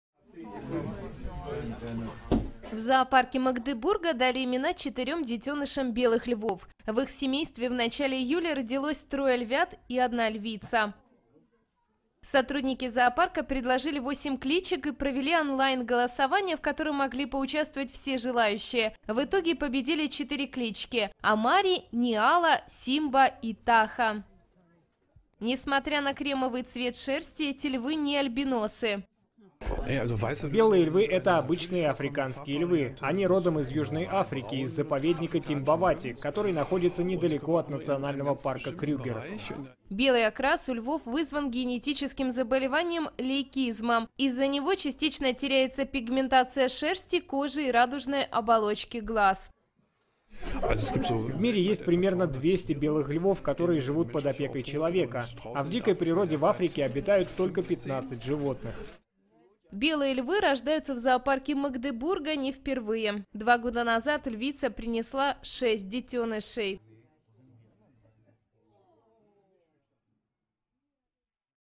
Как назвали белых львят в зоопарке Магдебурга
В магдебургском зоопарке дали имена четырём белым львятам. Какие - давайте узнаем.